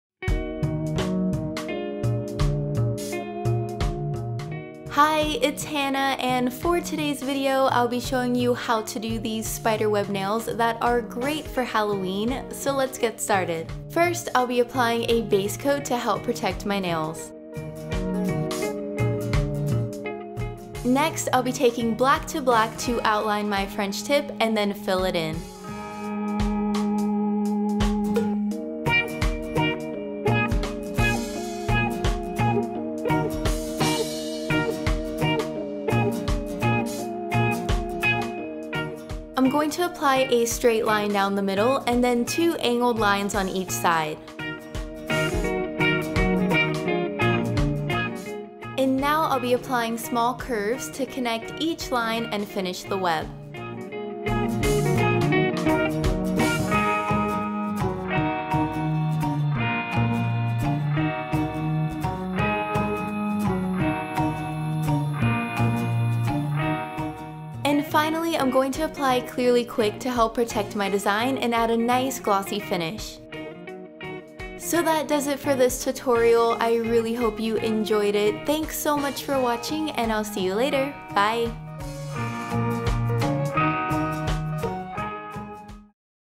Easy Spiderweb Halloween Nail Art Tutorial by Hannah Rox It
This negative space spiderweb nail art makes creepy crawlies seem cute! Follow Hannah Rox It’s step-by-step for an easy Halloween manicure using Insta-Dri in the Black to Black shade.